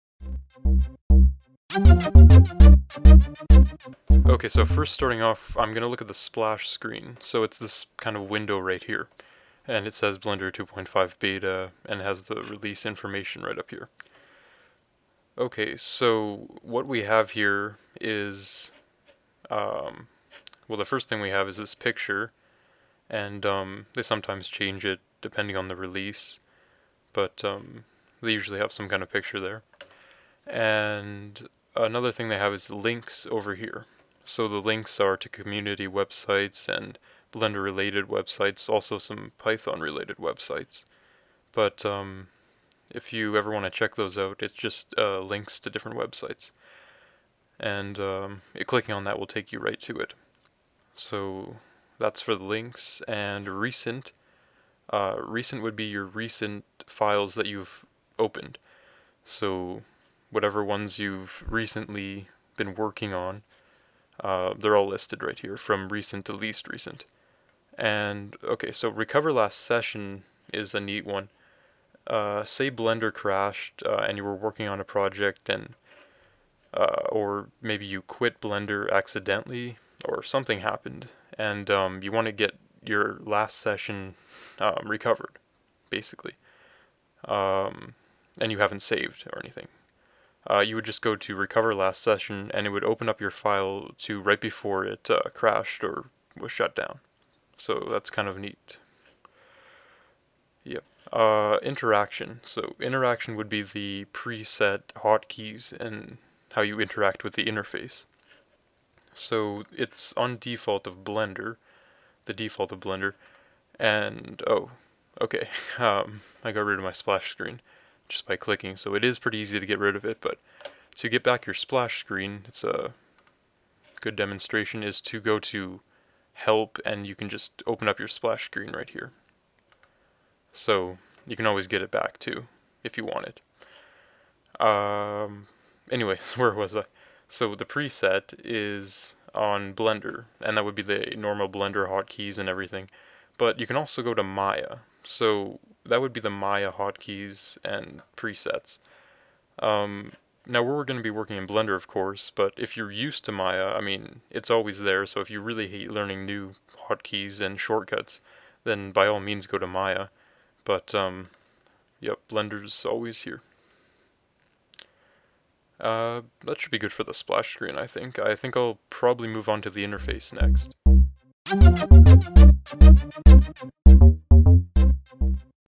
Blender Basics 1. Splash Screen
The first tutorial in the blender basics series. About the splash screen and its features.